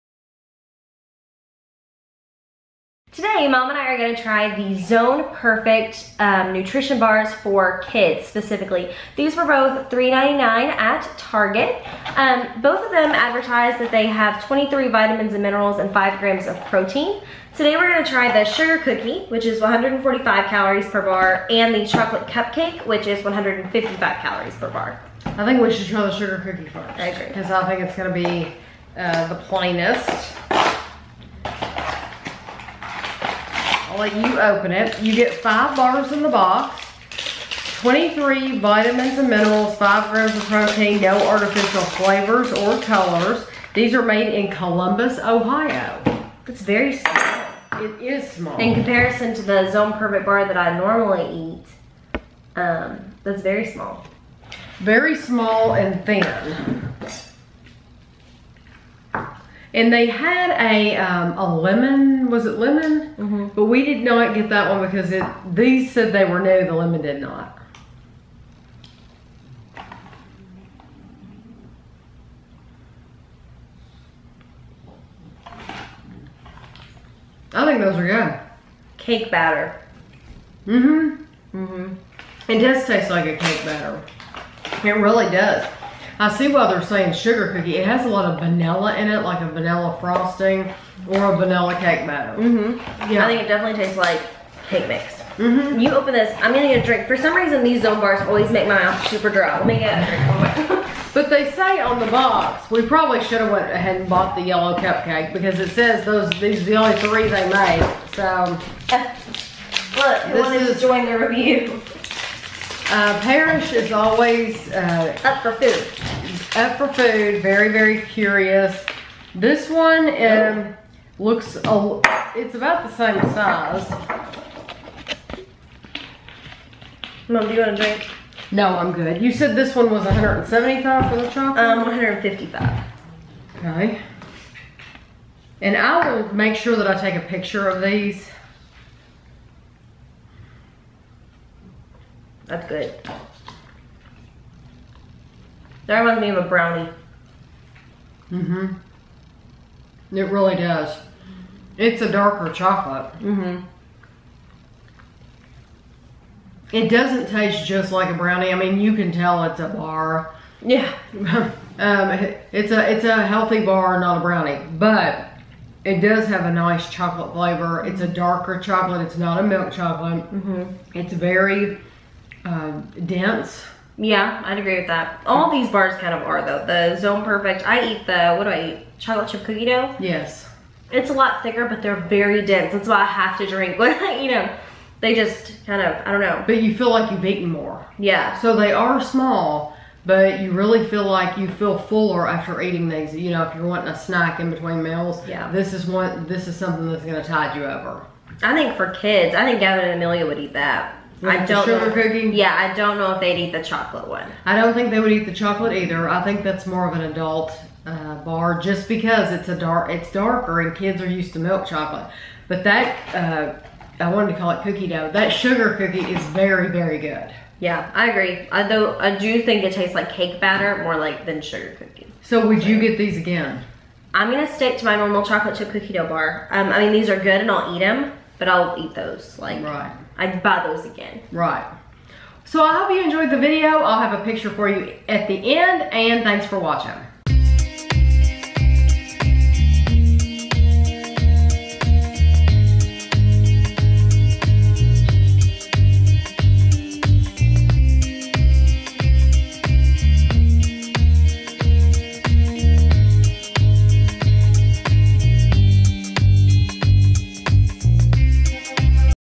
Zone Perfect Kidz Nutrition Bars: Sugar Cookie and Chocolate Cupcake Review
This is a taste test/review of the new Zone Perfect Kidz Nutrition Bars in the Sugar Cookie flavor and Chocolate Cupcake flavor. These bars contain no artificial flavors or colors. There are 5 bars in each box. They were $3.99 per box at Target.
* Sugar Cookie 1 bar (35g) = 145 calories
* Chocolate Cupcake 1 bar (35g) = 155 calories